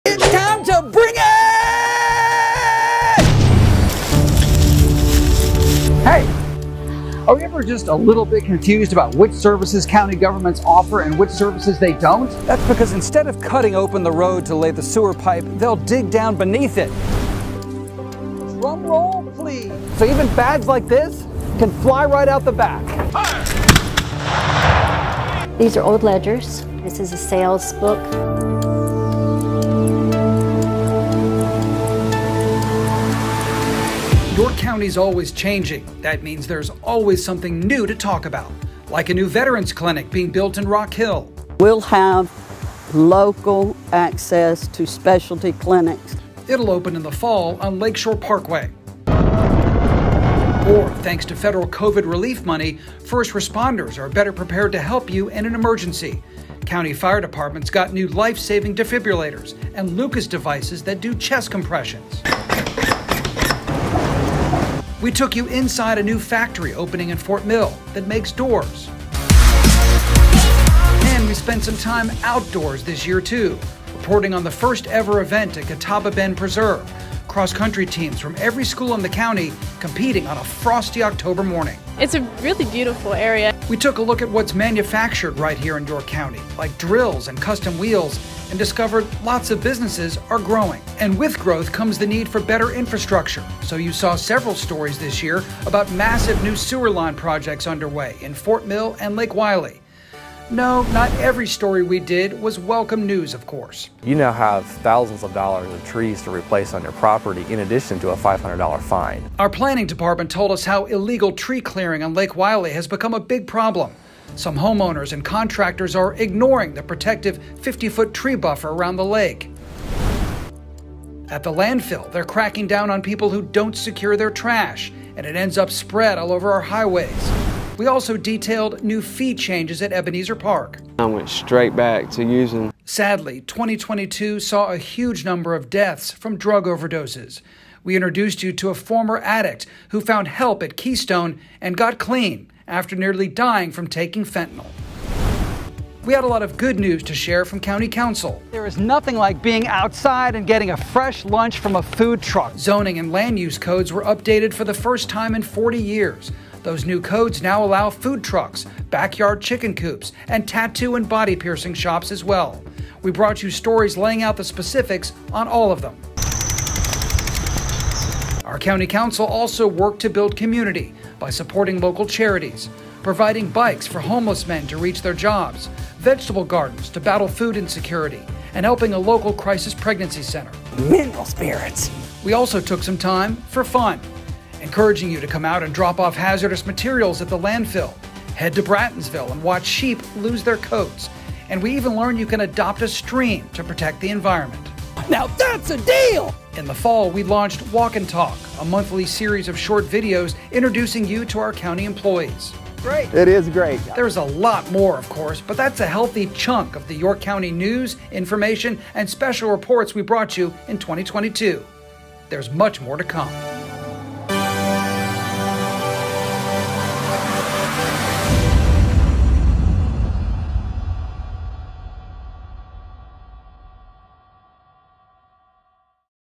END OF YEAR REVIEW PKG
Happy New Year everyone! (well almost). We brought you a ton of video content this year, and hopefully some good information too! Take a glance at some of the stories, events, and ideas we shared with you in 2022.